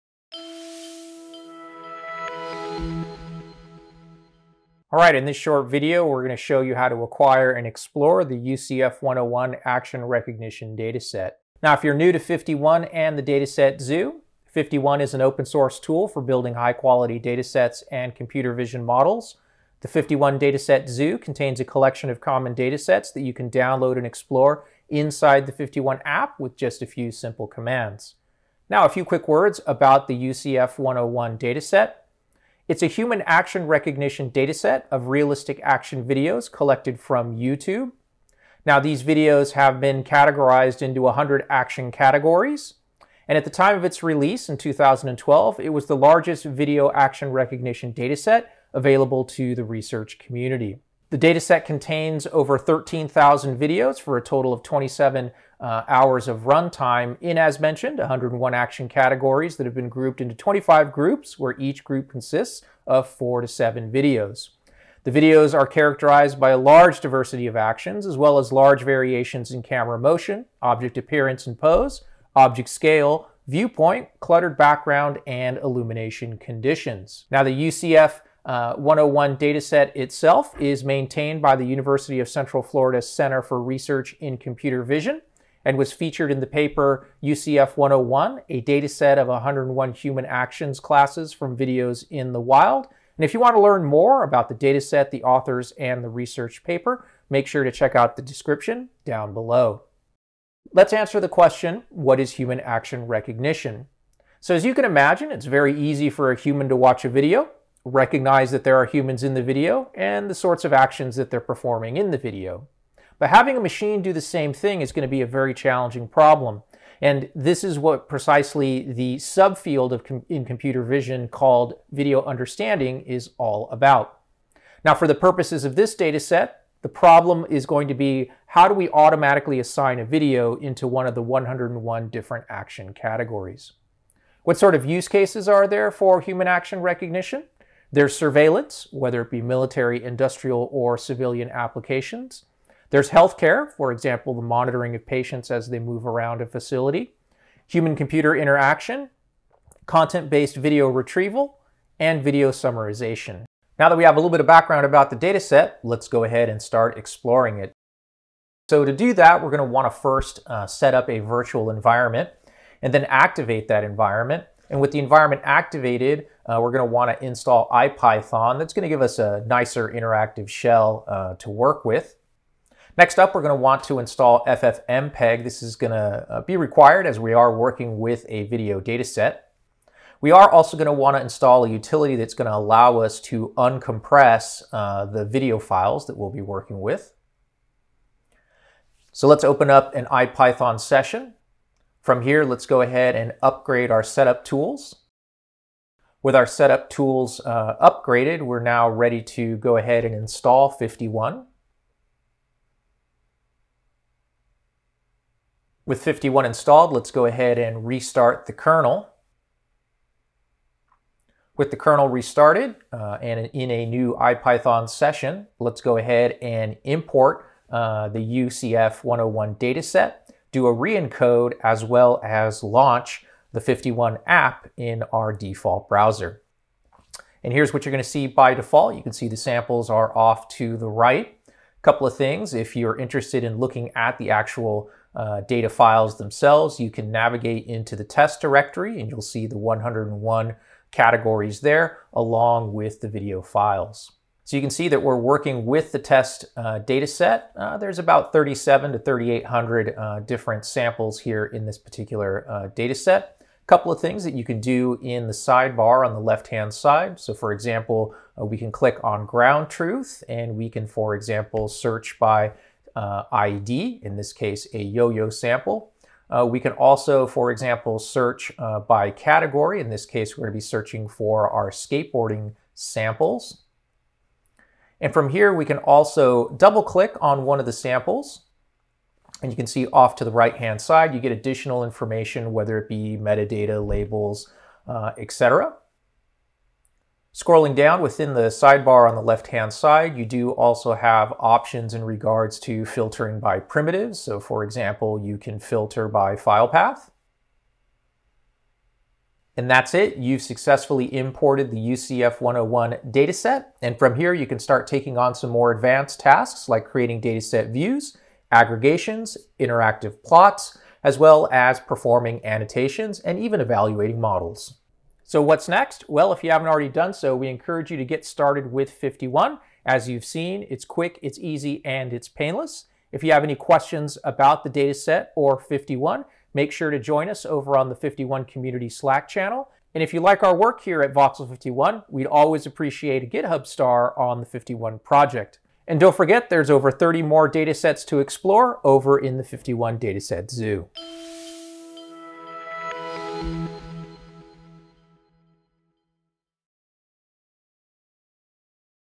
FiftyOne Dataset Zoo: UCF101 YouTube-Based Action Recognition Dataset
In this short video we cover:

* What is the UCF101 dataset?
* What is human action recognition?
* How to install the open source FiftyOne computer vision toolset
* How to explore the basic characteristics of the dataset